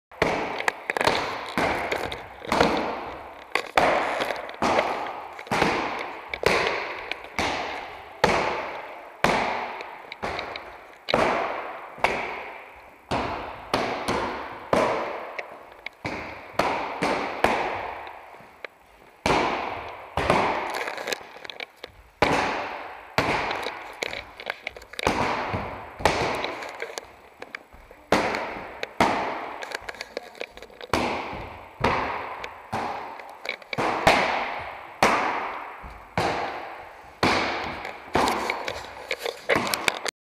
Двоечка Лоу кик (Маваше гери гедан) на длинной дистанции Клуб «Тэнгу Про» Мурманск
Двоечка Лоу кик (Маваше гери гедан) на длинной дистанции
Даша стаж - 2 месяца, Лёня стаж полгода